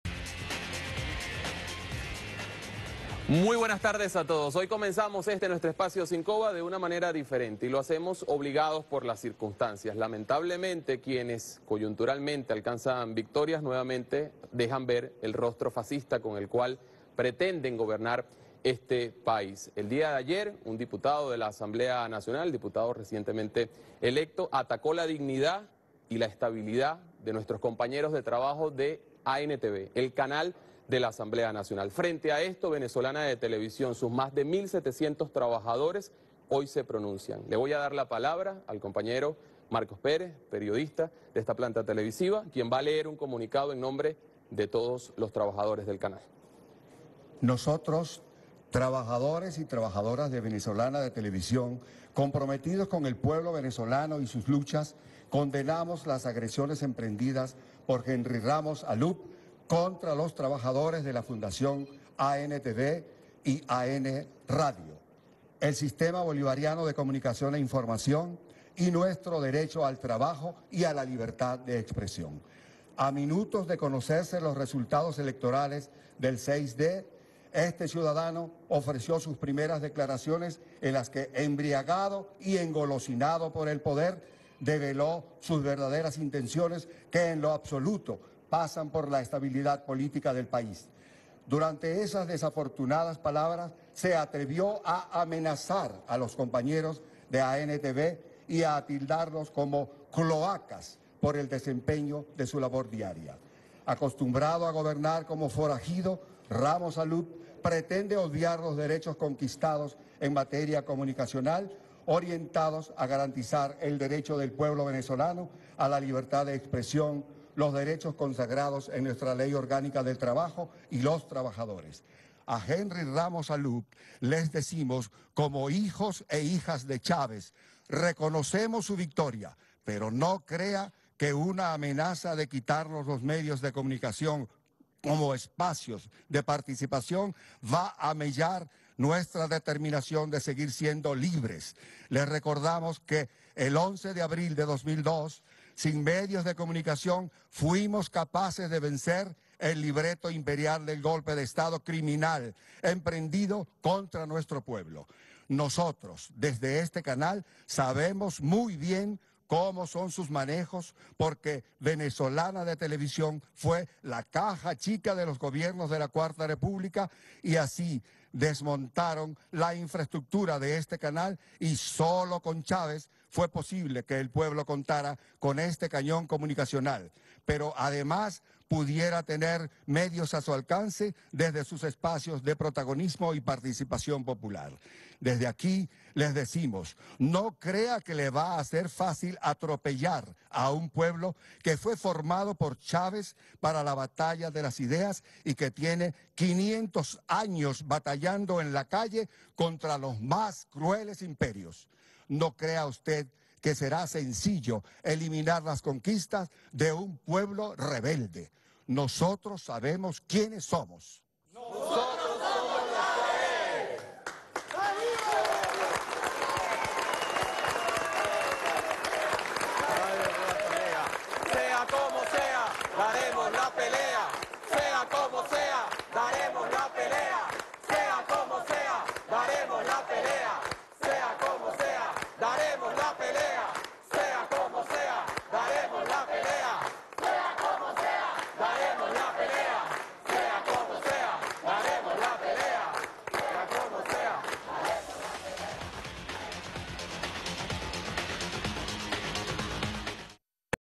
Trabajadores de VTV se solidarizan con ANTV tras amenazas de Henry Ramos Allup
Comunicado emitido este 7 de diciembre de 2015